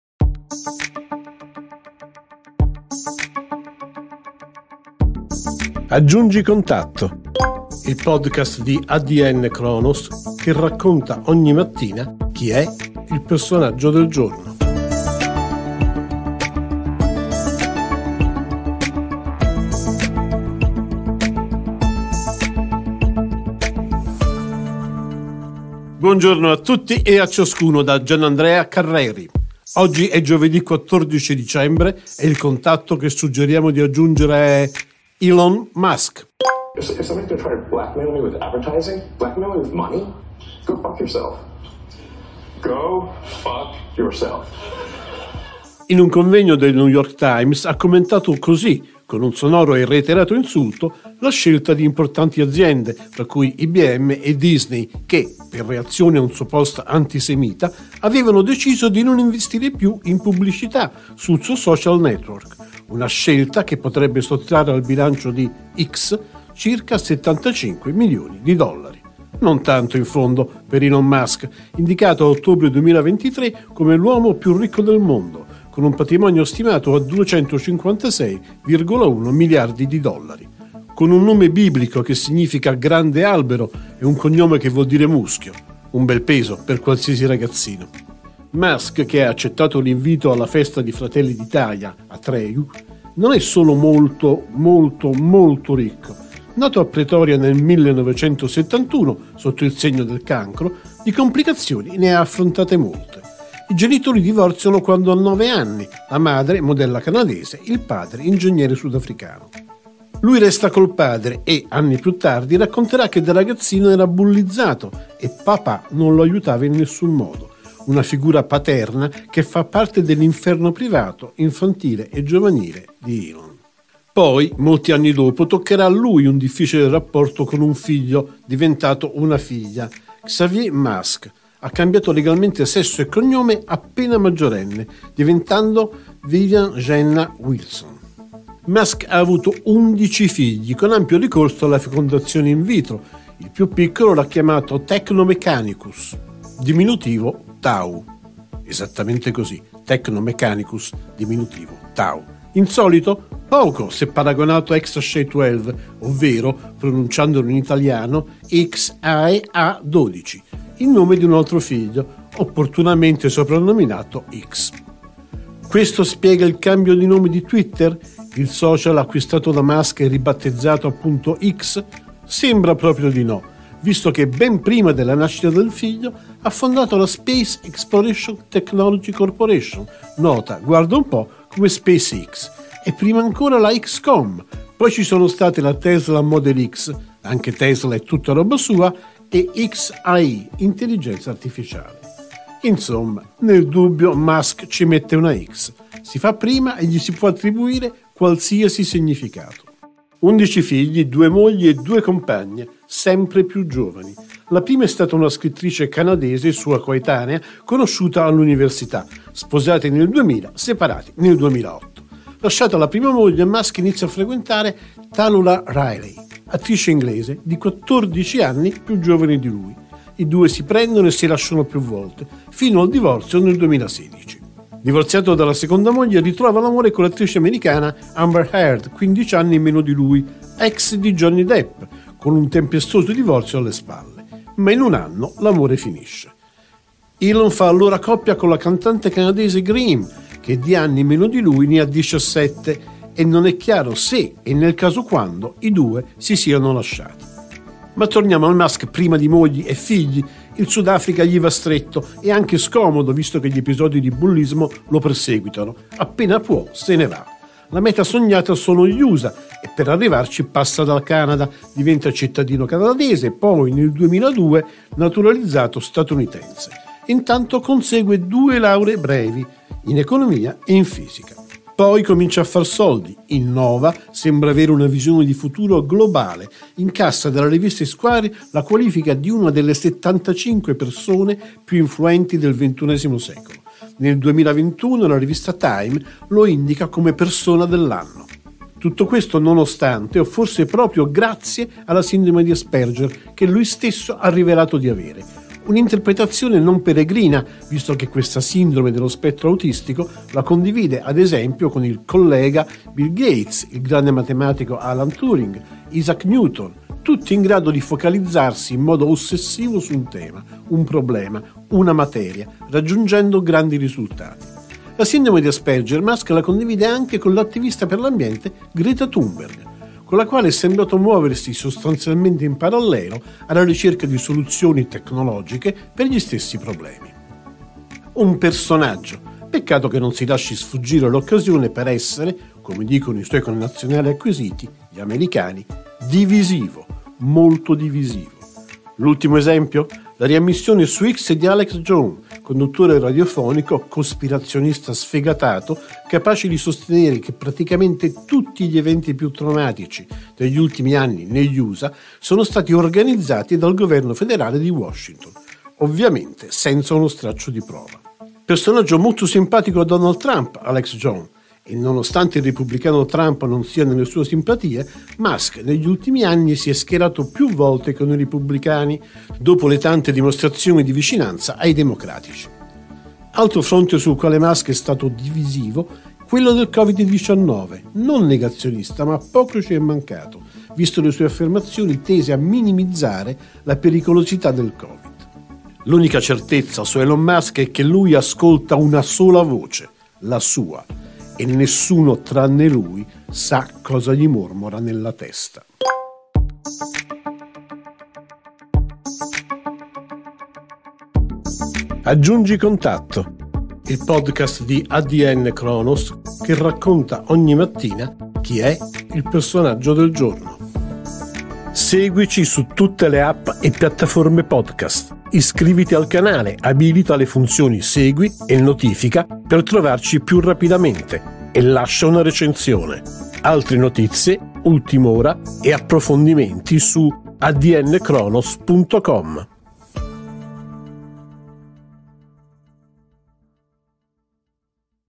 Elon Musk, non solo ricco - Aggiungi contatto - Podcast
Elon Musk è l’uomo più ricco del mondo, e uno dei più divisivi, ha la sindrome di Asperger, 11 figli, 256,1 miliardi di dollari, un’infanzia faticosa e, da sempre, grandi progetti che sono diventati grandi società. Sarà super ospite alla prossima edizione di Atreju, la festa di Fratelli d’Italia.

Immagine: Fotogramma
Musiche su licenza Machiavelli Music.
AdKey:zP-94qNWQqj3XM